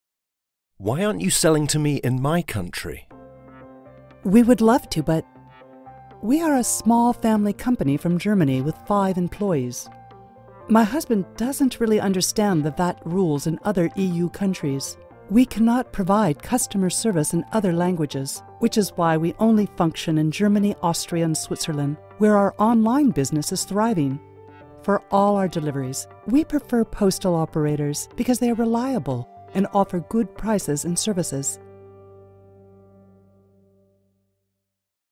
EP01: Challenges in E-Commerce
The main obstacles SMEs face before placing their products online are: language barriers, lack of digital skills, complying with various Consumer Rights and VAT regimes in destination countries, etc.